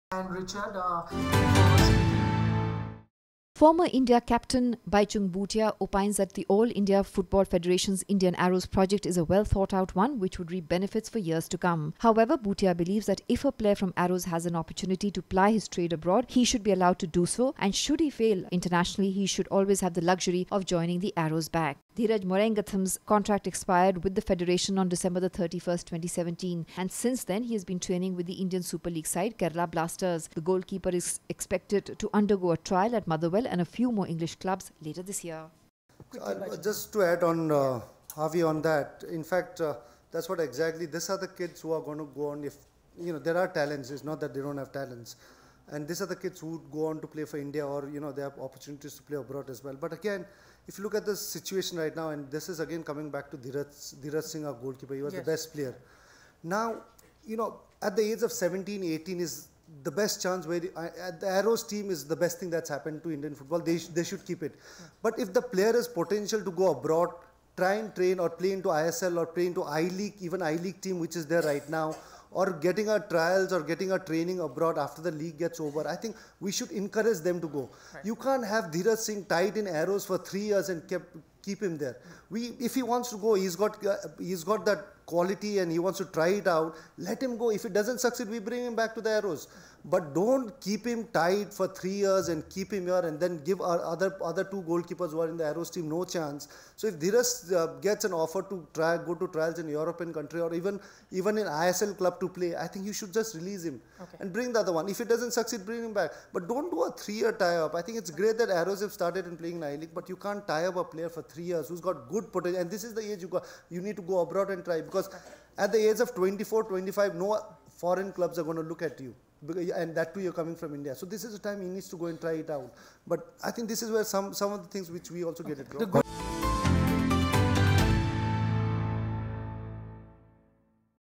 Dheeraj Should Have Been Encouraged To Go Abroad- Bhutia
Former Indian captain Bhaichung Bhutia opines that the All India Football Federation’s Indian Arrows project is a well thought out project which would reap benefits for years to come.
However, Bhutia believes that if a player from Arrows has an opportunity to ply his trade abroad, he should be allowed to do so and should he fail abroad, he always should have the luxury of joining the Arrows back.